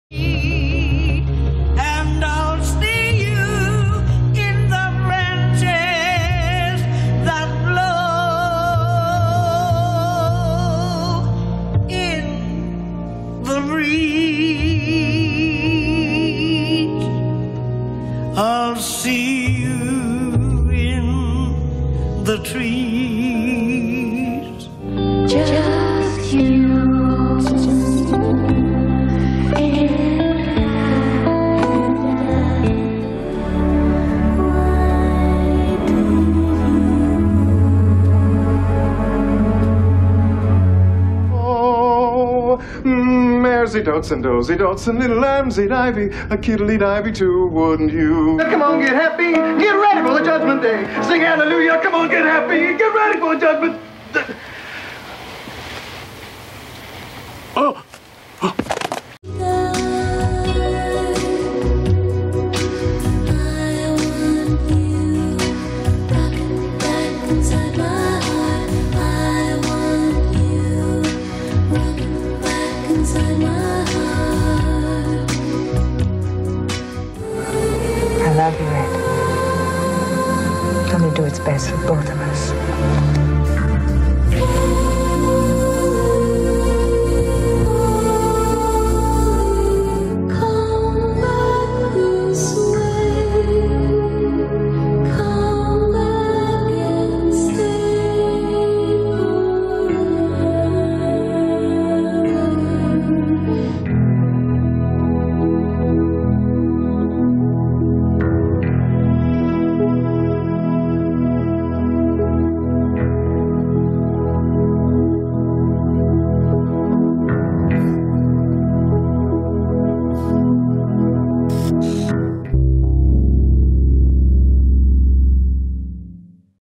Songs in the Key of Lynch
With the revival of TWIN PEAKS, we explore David Lynch's use of song and music to reveal his characters' emotions and deepest secrets.